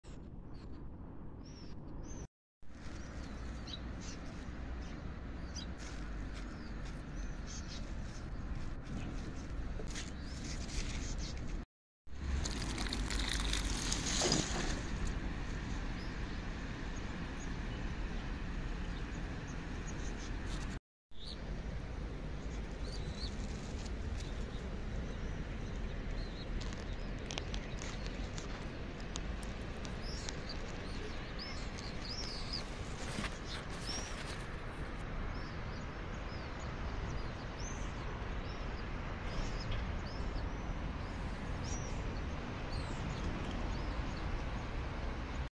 How to grow organic courgette in pot
Urban Gardening , easy way to grow your own vegetables at home , in banlcony , terace just in pot s